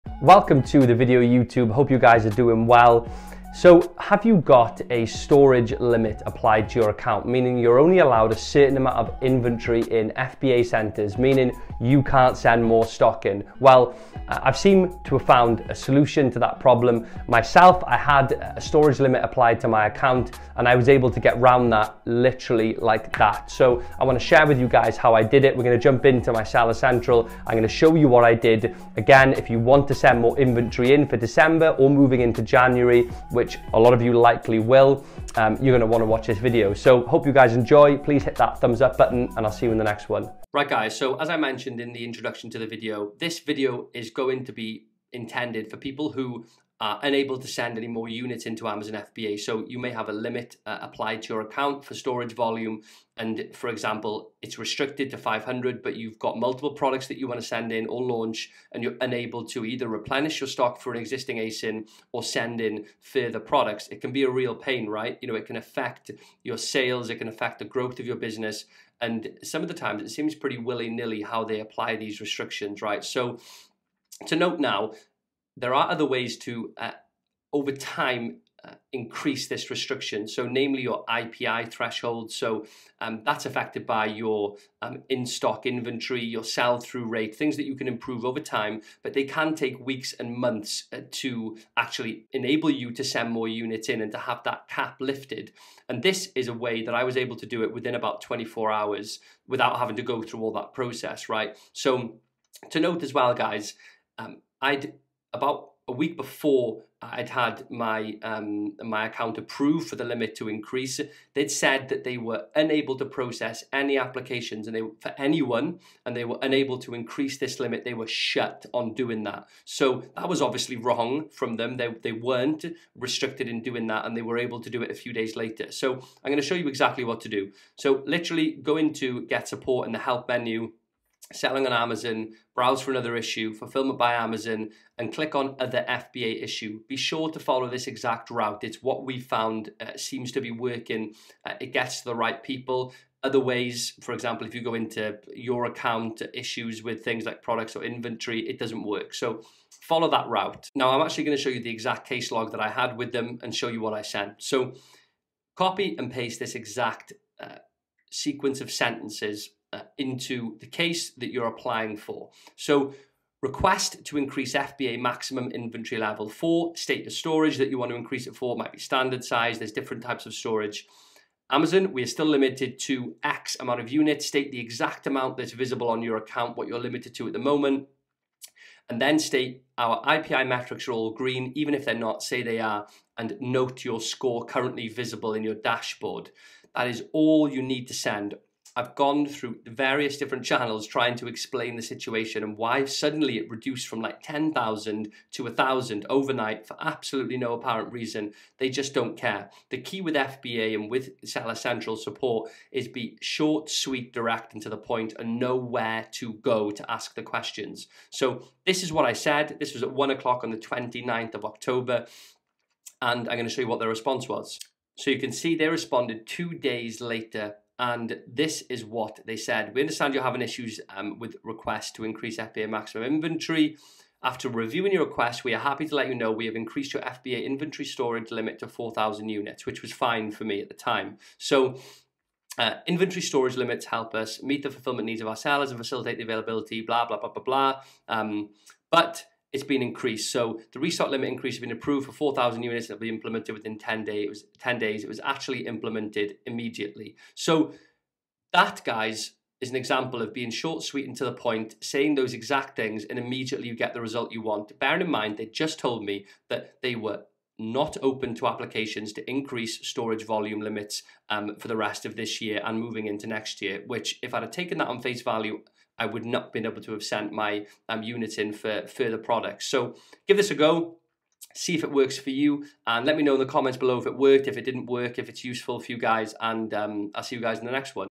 How To Increase Your Amazon FBA Storage Limits WITH ONE CLICK (Quick Fix Demo)
⭐ The ECOM-HUB PRO 2025 - Full Mentorship and Training (Limited Spaces)

How to increase your Amazon FBA Storage Limits Quickly.